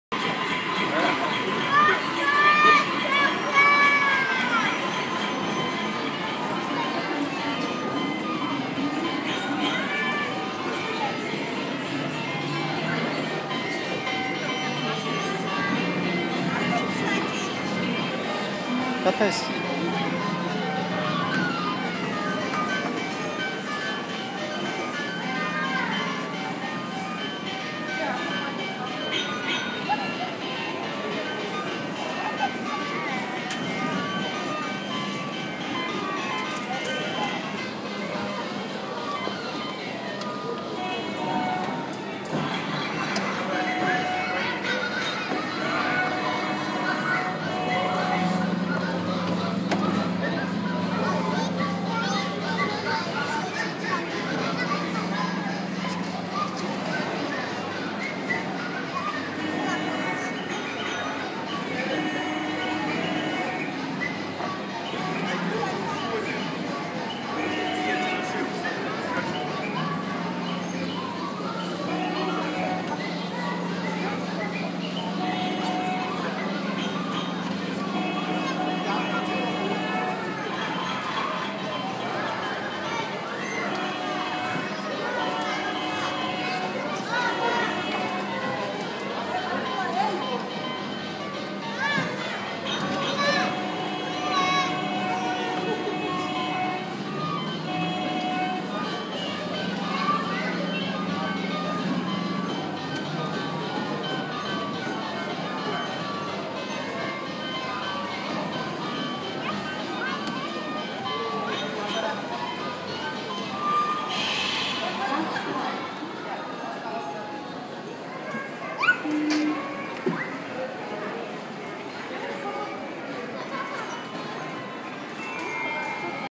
Детский аттракцион Лондонский Автобус
С ветерком прокатились на чудо-аттракционе "London Bus"

Детский аттракцион «London Bus» — очень яркий и заметный аттракцион, который всегда притягивает взгляды людей в торговых центрах или парках!